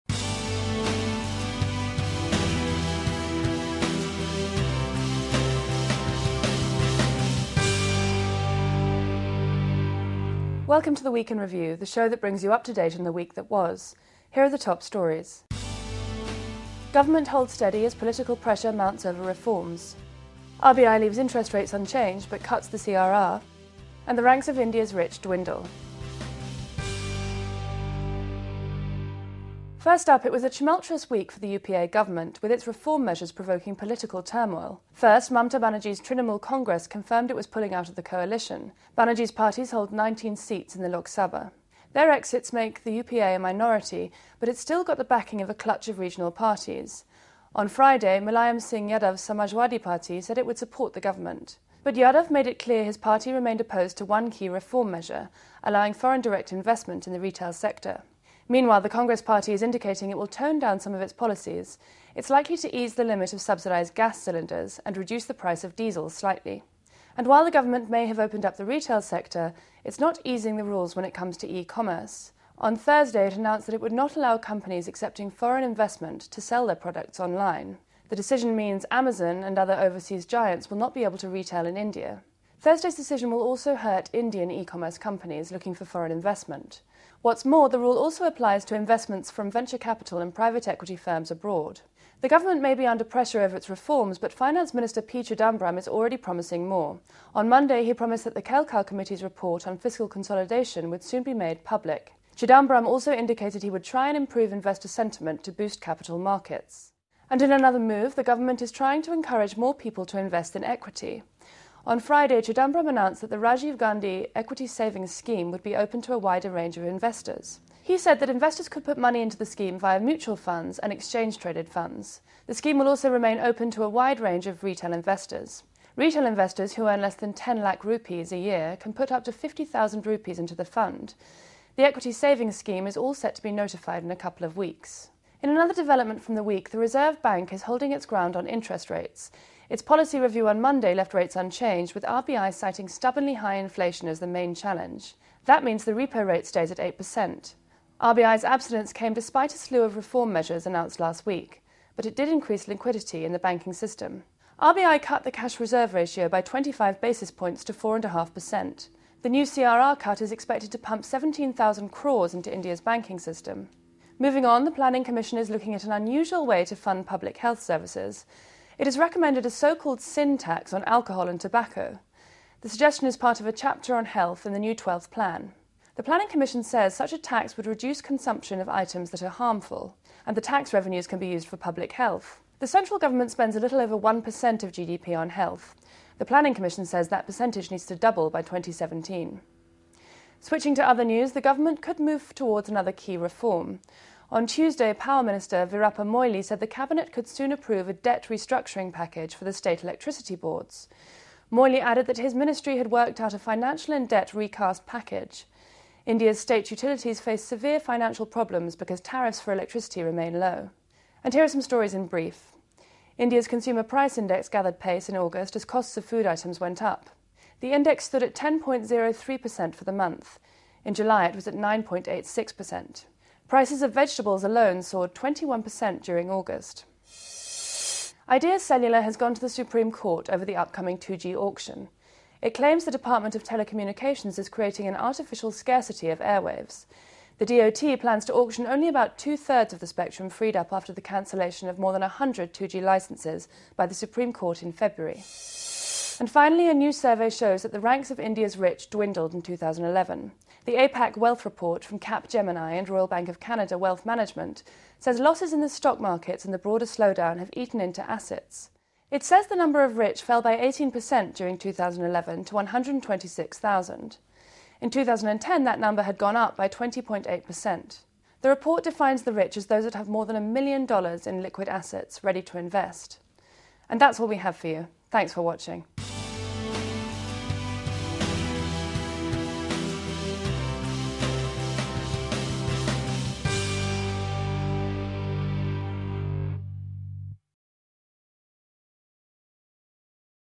The Week in Review
Your executive summary of the top business stories of the week, from the state of the government's reform measures to RBI's monetary policy.